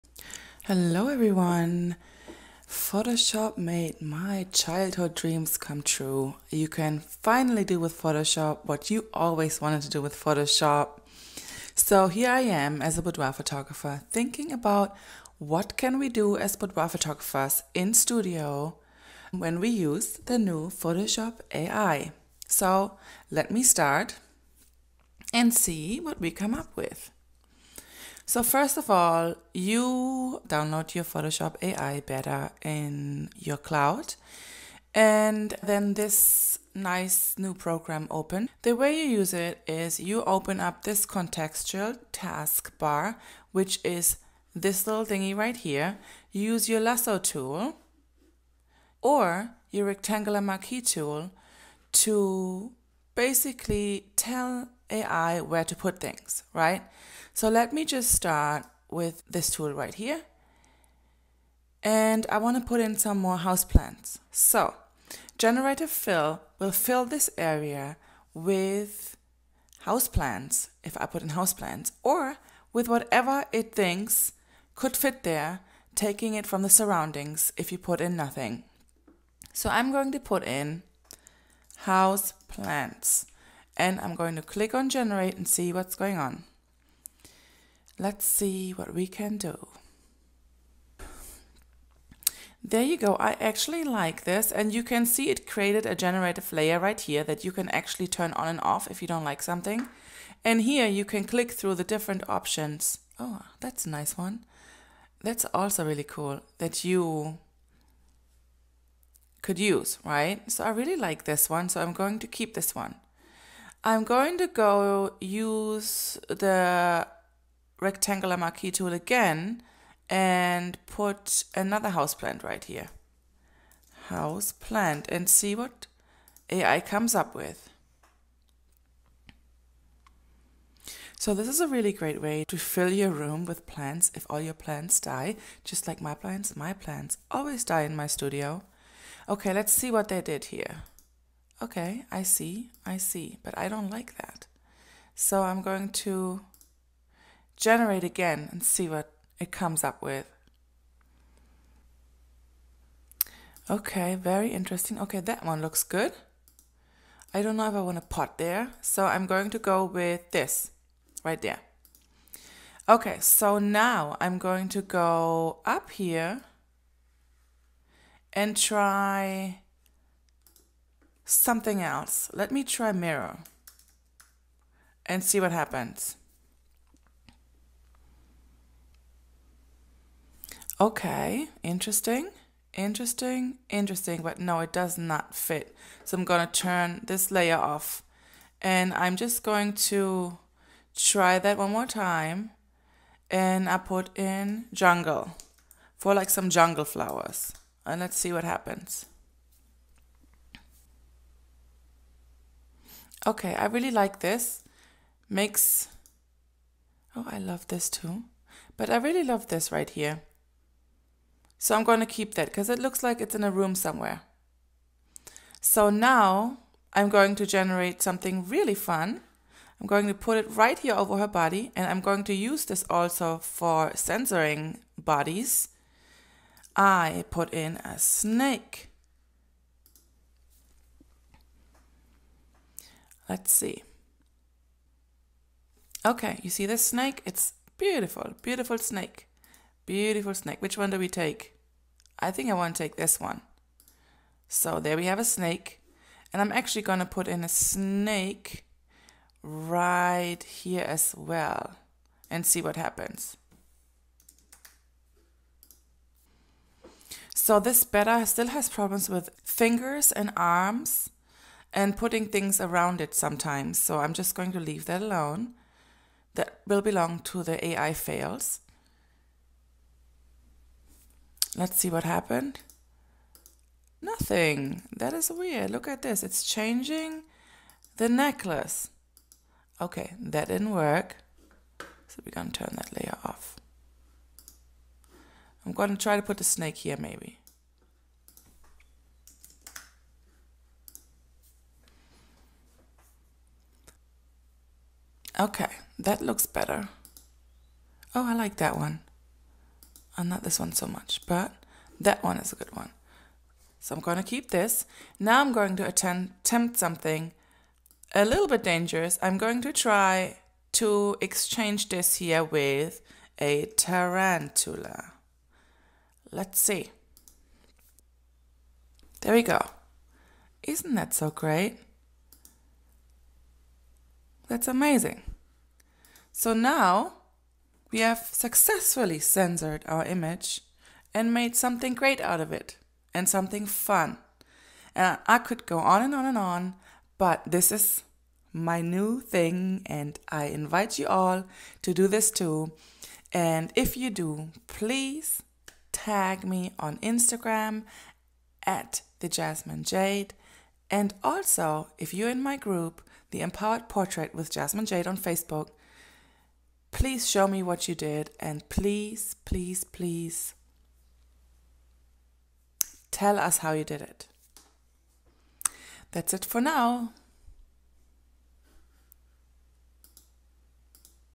How to use Photoshop Beta Ai
How can we use the new Photoshop Beta Ai for indoor photography especially portraits and boudoir.
Well I came up with some fun things!
Prompted House plants, snakes and a Tarantula !
This is how you can use the new photoshop Beta to enhance your photography.

if you watched this video and start on some fun edits!